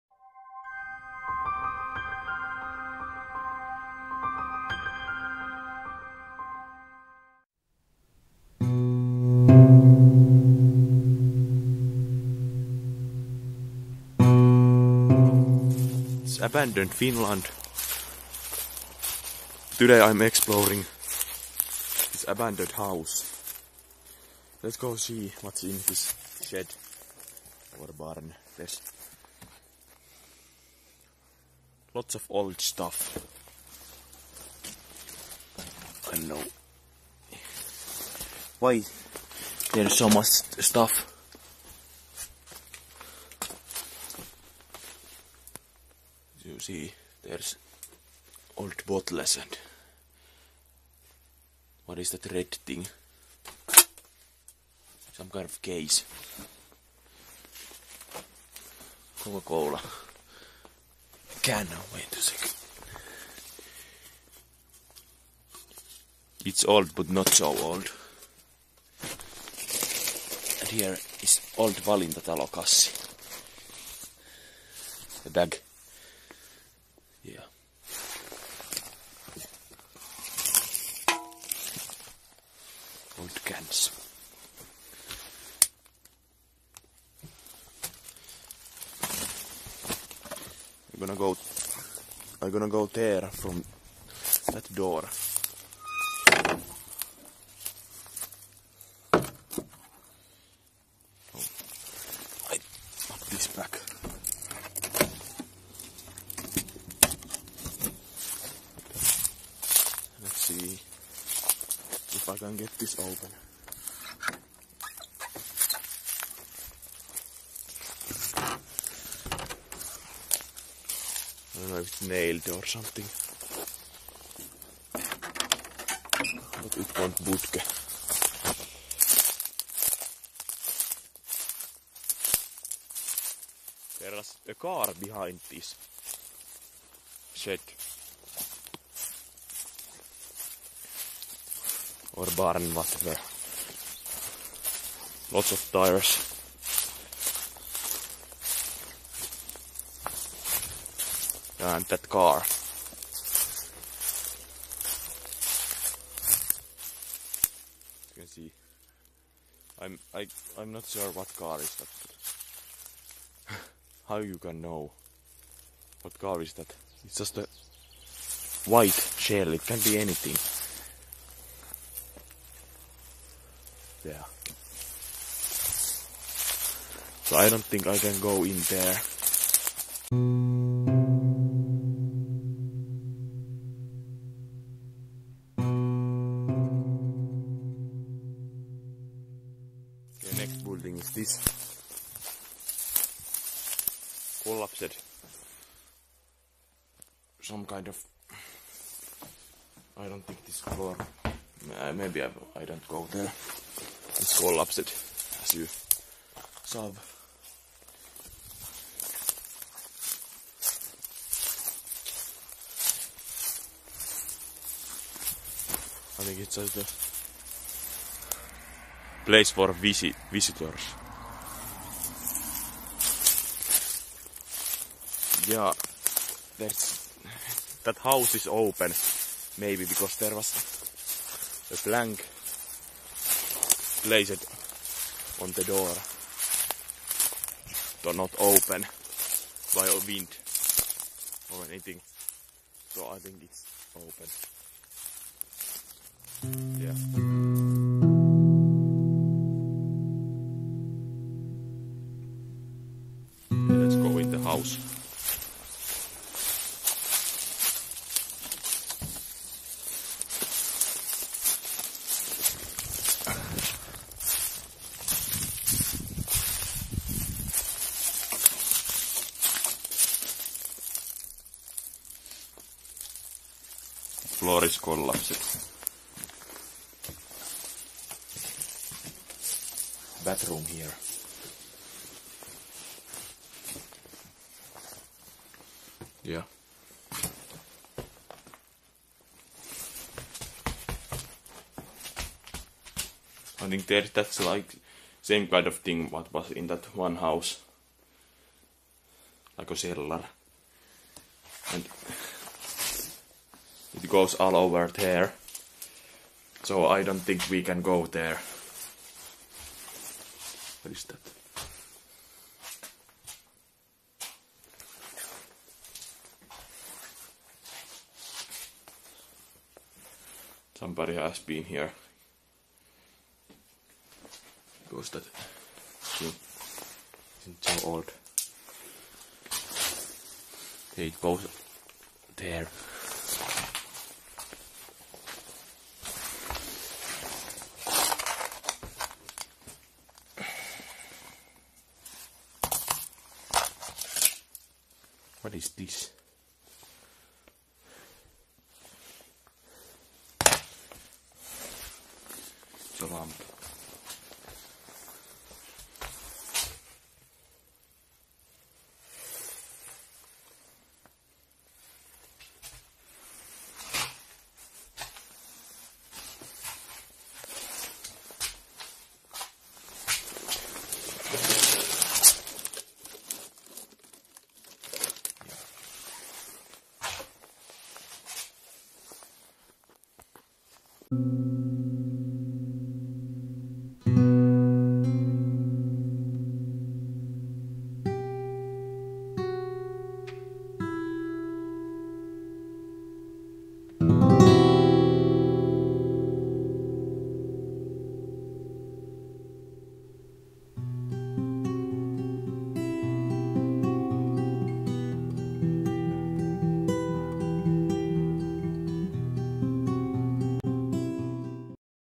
Exploring abandoned house in Finland - Lots of old stuff
I explored this abandoned house in Porvoo, Finland. There was lots of old stuff in the shed and a car behind it.